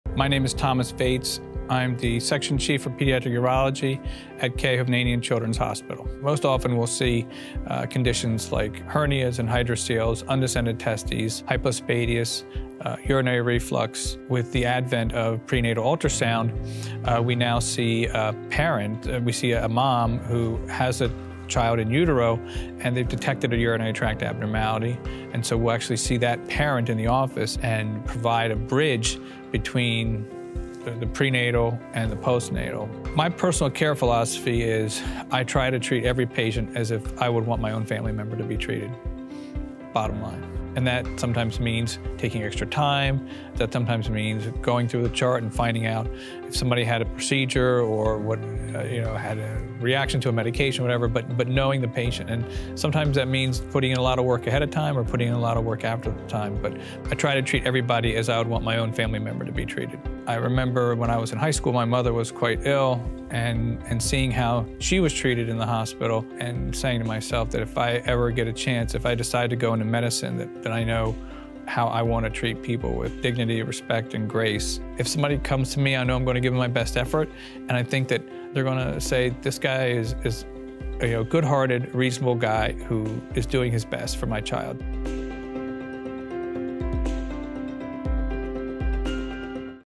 Thomas Vates, M.D. - About the Provider
Thomas Vates, M.D. is the Division Chief of Pediatric Urology at K. Hovnanian Children’s Hospital at Jersey Shore University Medical Center and Co-Director of Pediatric Urology
Hackensack Meridian Health Network. He introduces himself, his speciality, and his philosophy of care.